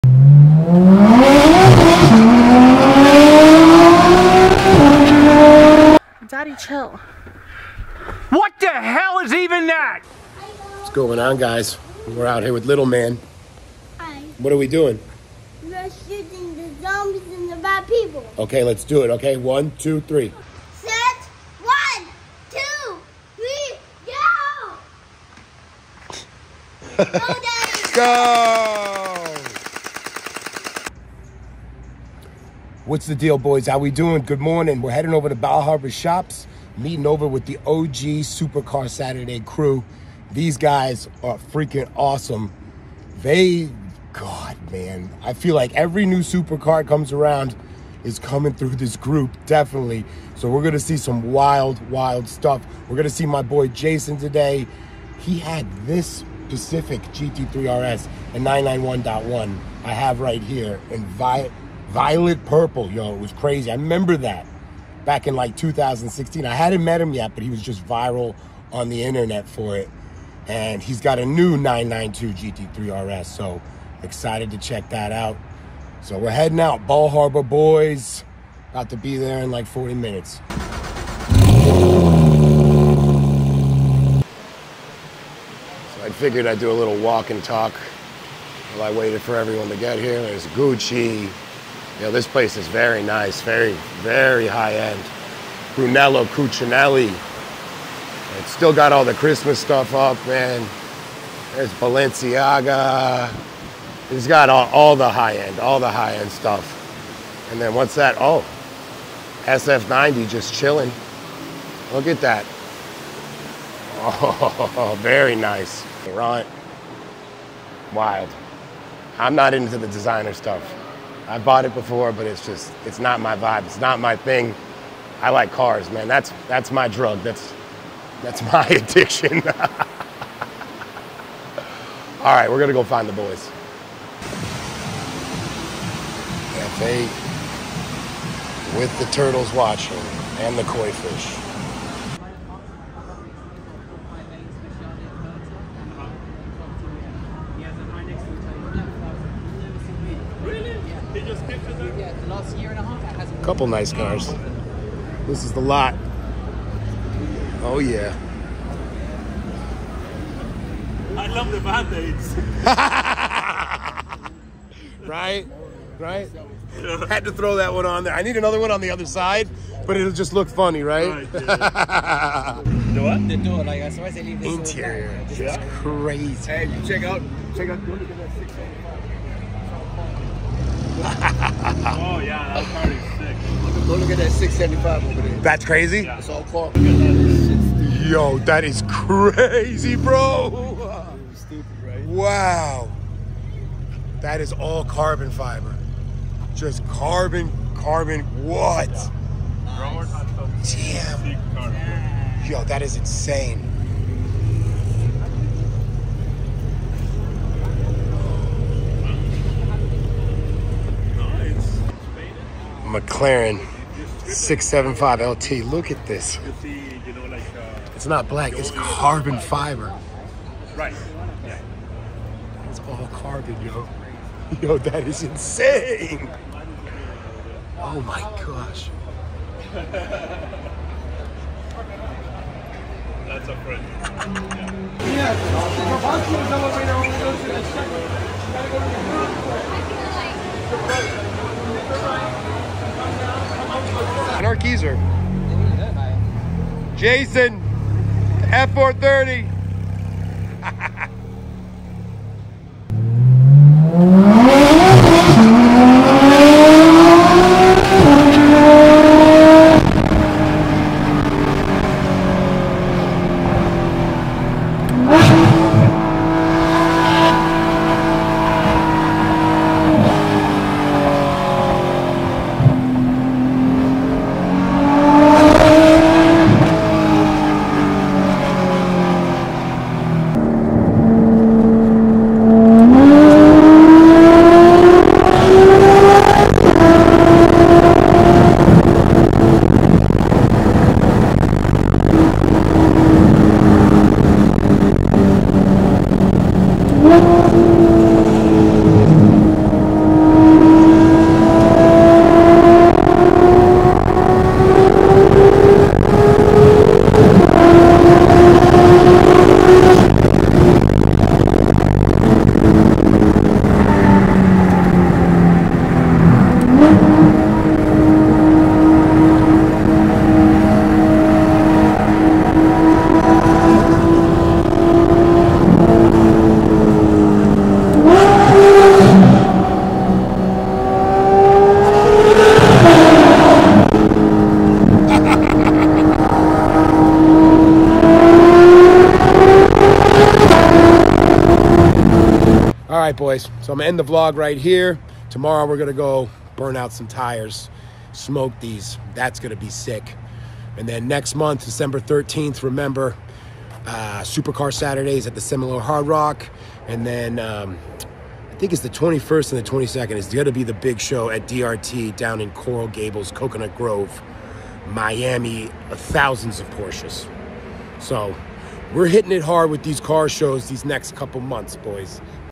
Bal Harbor Shops w/ the SCS Crew & GT3RS POV . DADDY CHILL !!!!!!
Took the Gt3RS to Bal Harbor Shops to meet up with the OG Super Car Saturday Crew